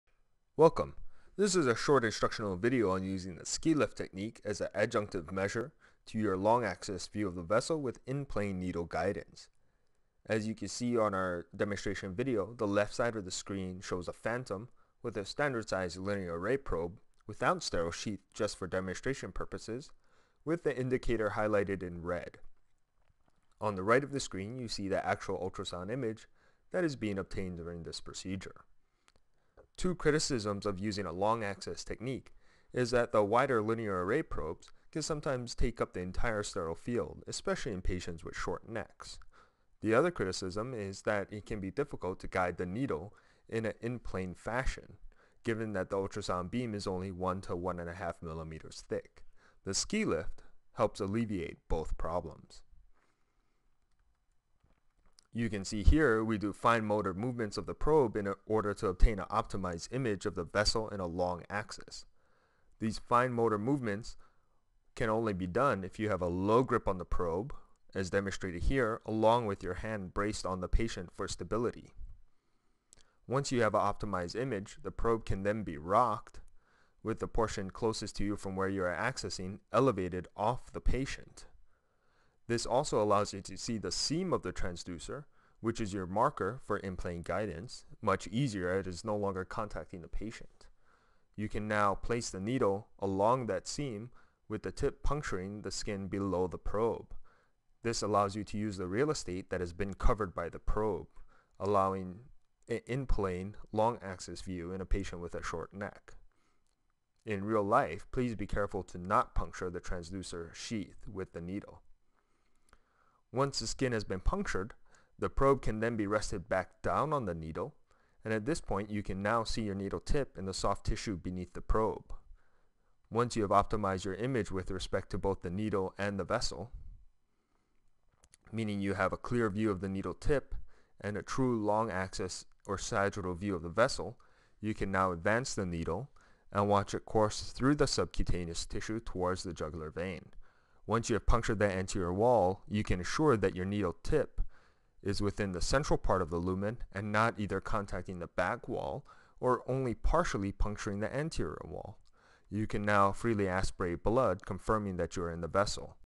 Ski Lift: a method to help in plane ultrasound needle guidance
Video describing the Ski lift technique, a method to help with alignment for ultrasound needle guidance.  This was originally published in AEM.